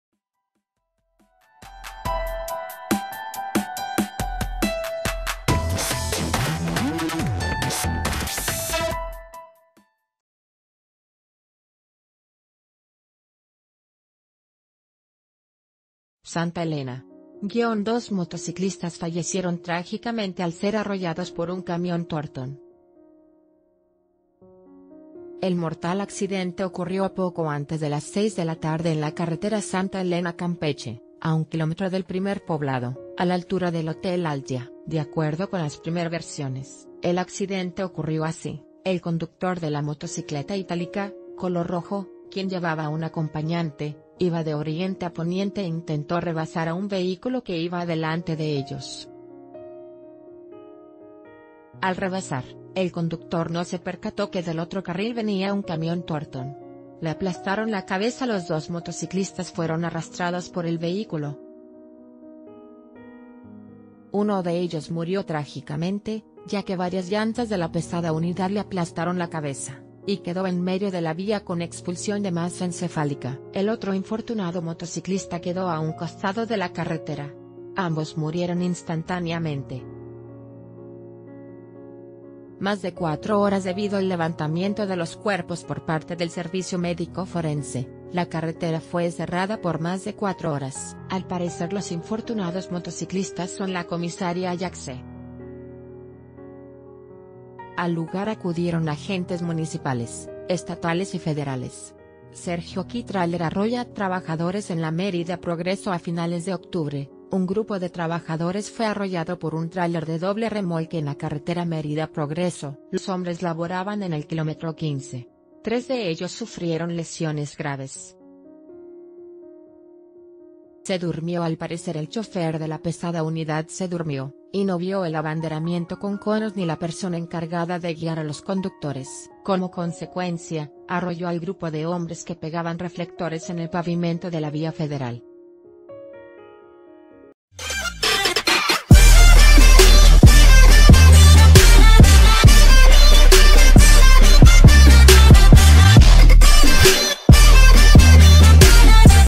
Dos personas mueren arrolladas por un camión torton - El Diario de Yucatán
SANTA ELENA.- Dos motociclistas fallecieron trágicamente al ser arrollados por un camión torton. El mortal accidente ocurrió poco antes de las 6 de la tarde en la carretera Santa Elena-Campeche, a un kilómetro del primer poblado, a la altura del Hotel Altia. De acuerdo con las primer versiones, el a...